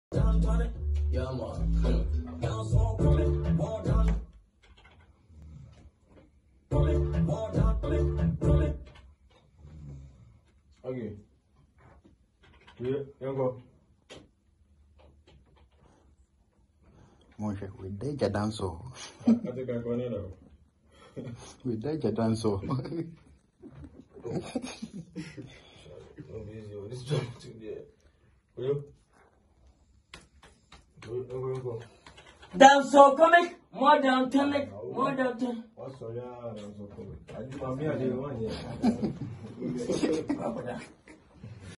New lil win dancehall
Behind the scene of lil win new dancehall with article wan